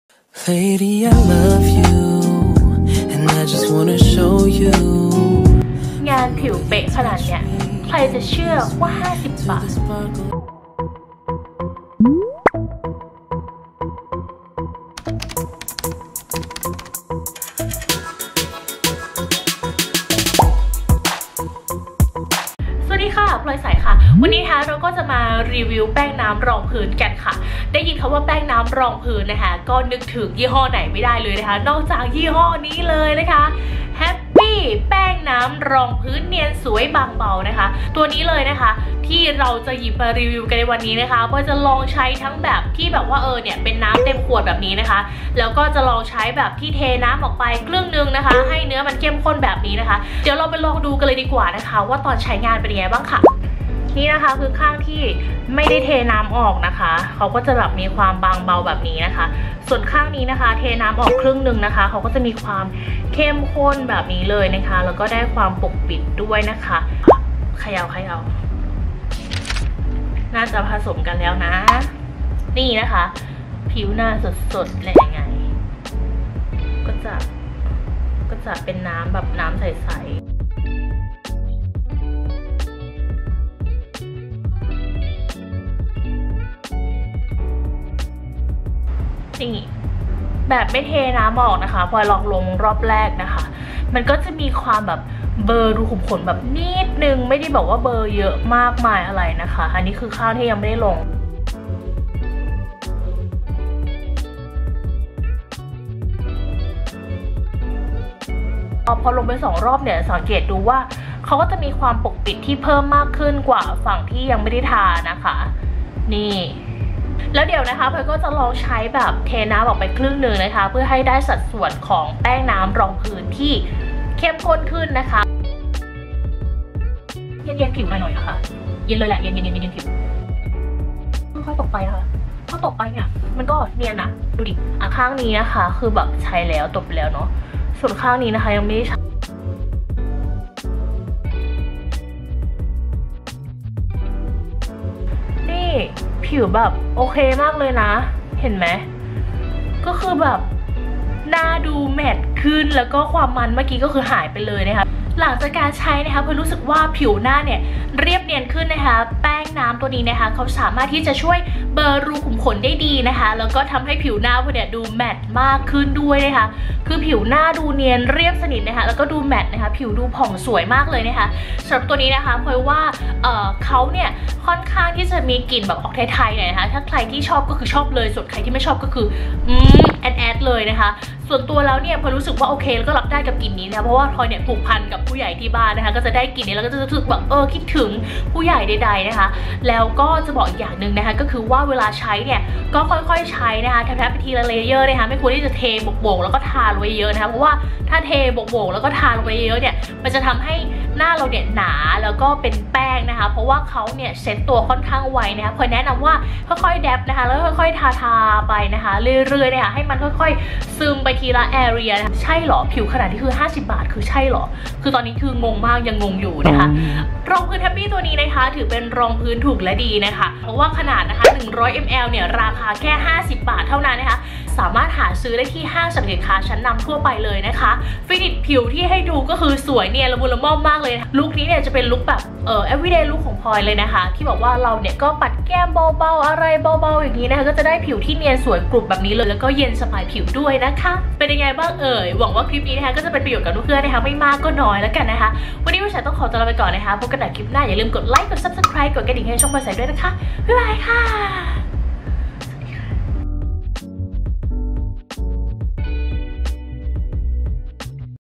Review : แป้งน้ำ Happyได้ลองสักที แป้งน้ำในตำนาน | PZployzaiiii
แป้งน้ำ Happy
Happy water-based foundation ขนาด 100 ml. ราคา 50 บาท

หากใครสนใจ ก็สามารถ หาซื้อได้ที่ 7-11, Tesco Lotus , Big C, CJ Express, Tops, The Mall, Family Mart และร้านค้าทั่วไป